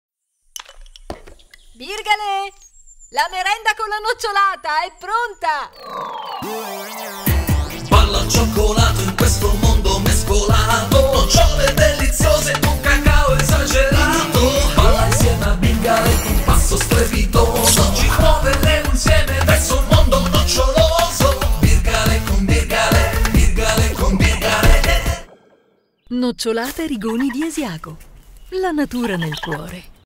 Rigoni di Asiago, spot Nocciolata con Birgale e Skiro
Lo Spot di Nocciolata Rigoni di Asiago che ha come protagonisti lo gnomo Birgale e lo scoiattolo Skiro.